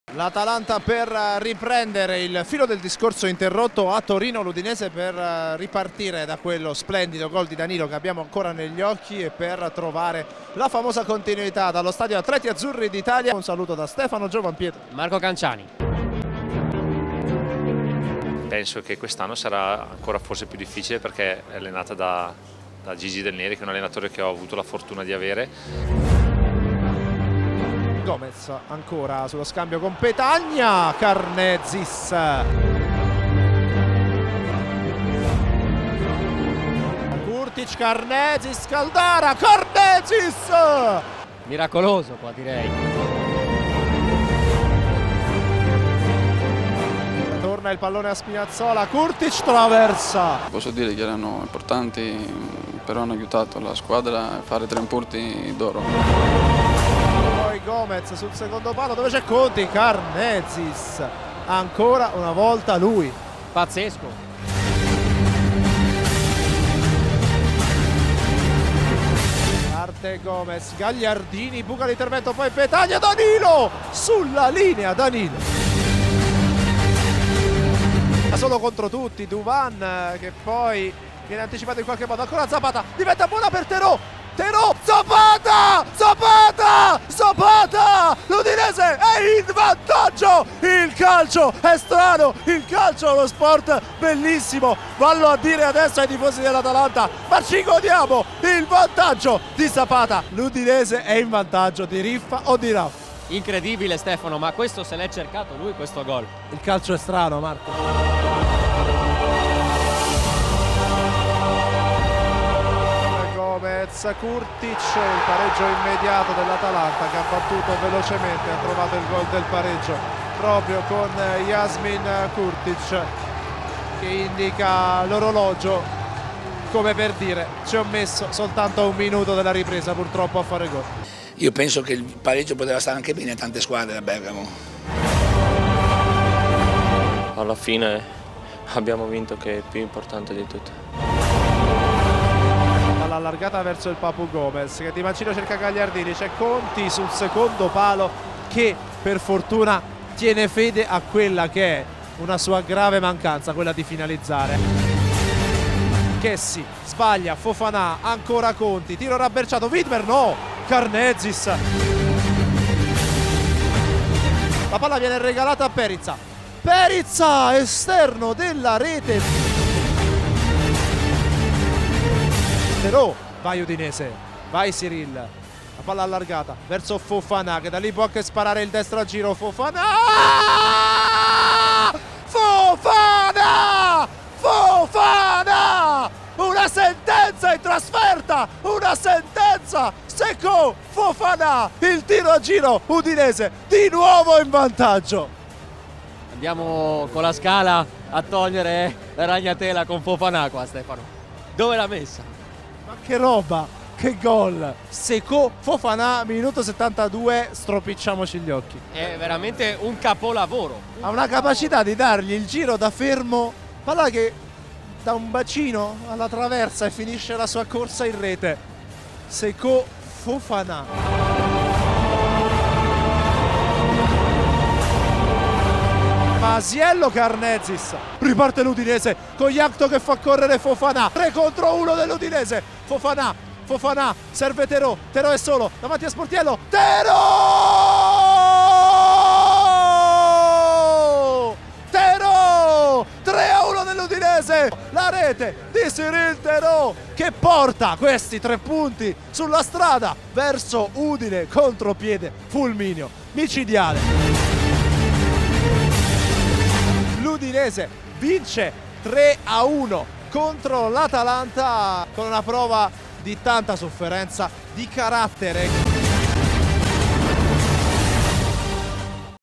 UDINESE TV - La sintesi musicata di Atalanta-Udinese 1-3
Rivivi le emozioni dell'Atleti Azzurri d'italia, con i gol di Zapata, Fofana e Thereau, nel commento di Stefano Giovampietro e Marco Canciani